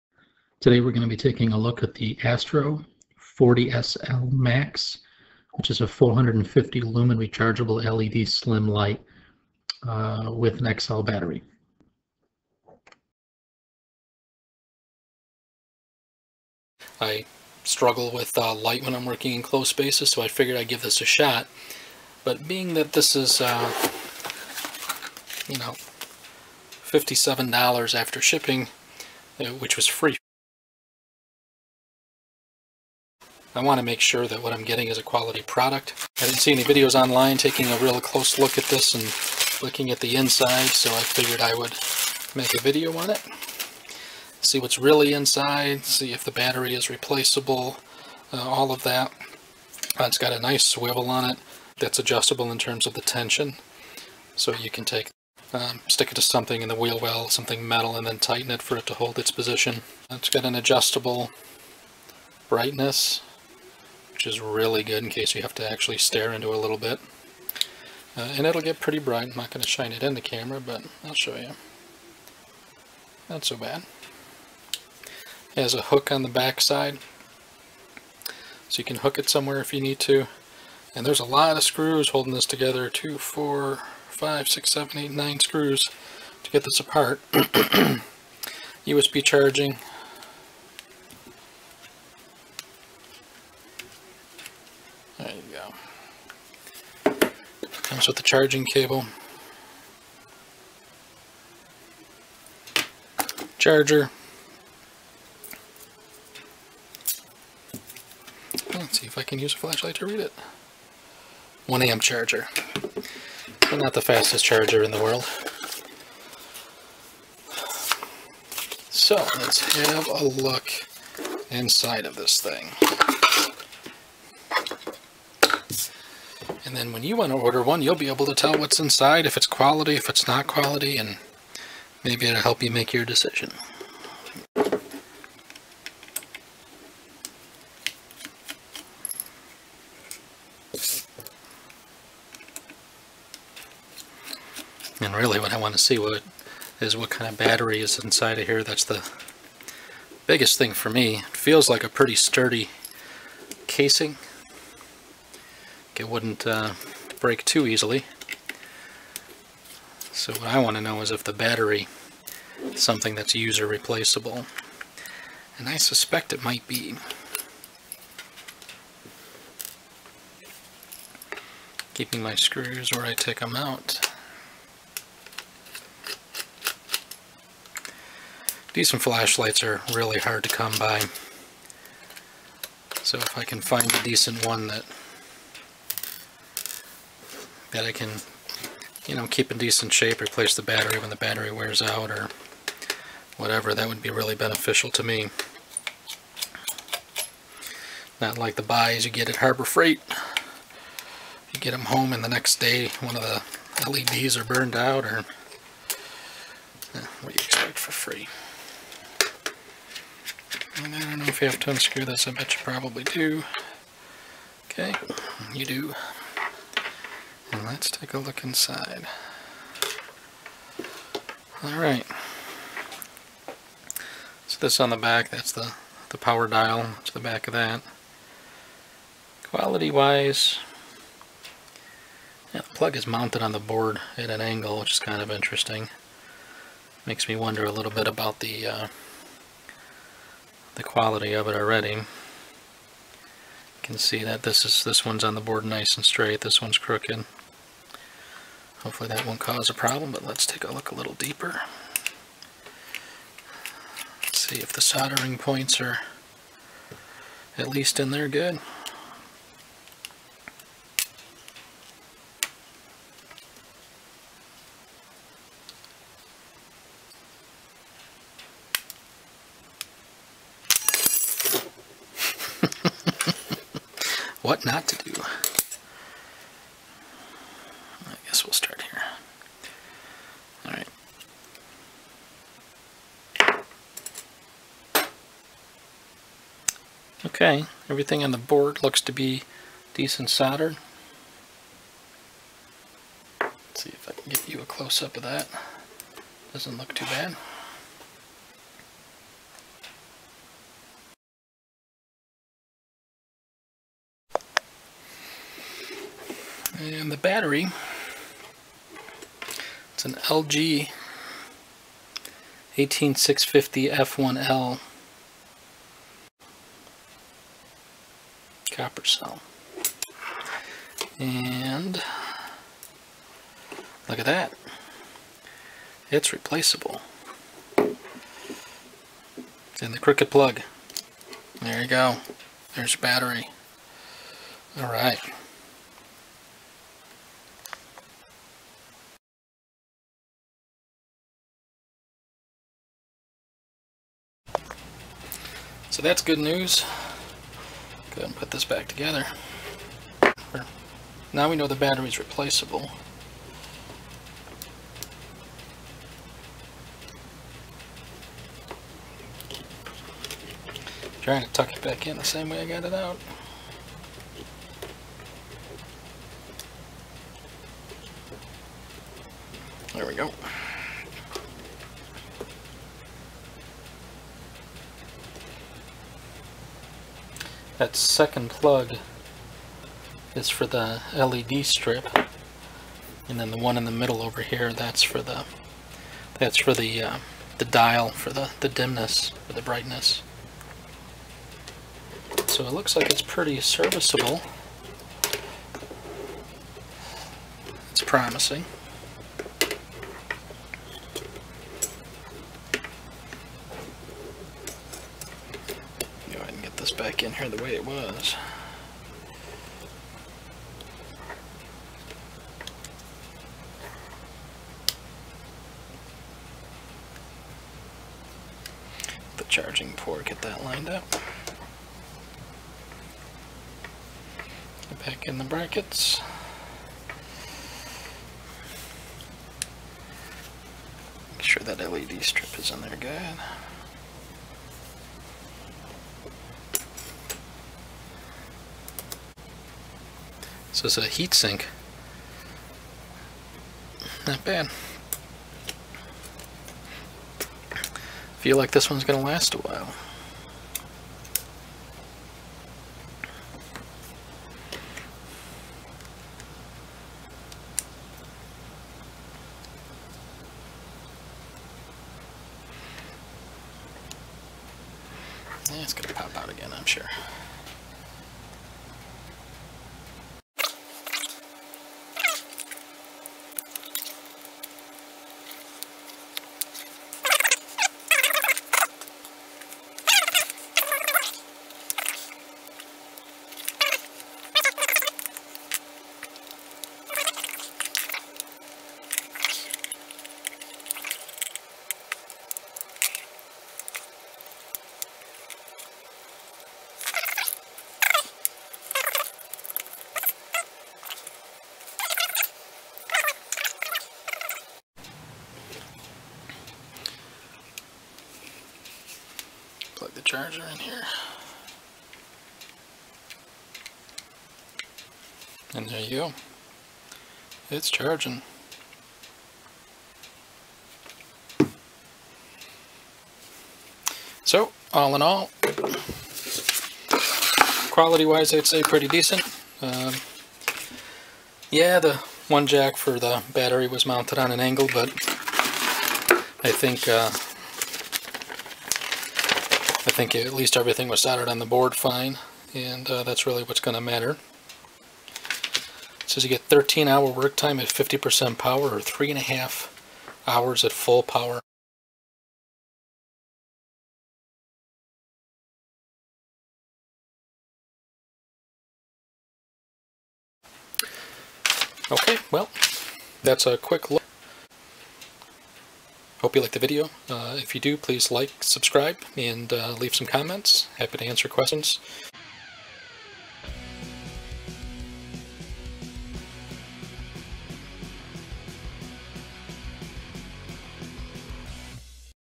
Astro 40SLMAX LED Light Review & Disassembly for Battery Change
Dissembling the Astro 40SLMAX LED light to change the battery.  Look inside and quality review.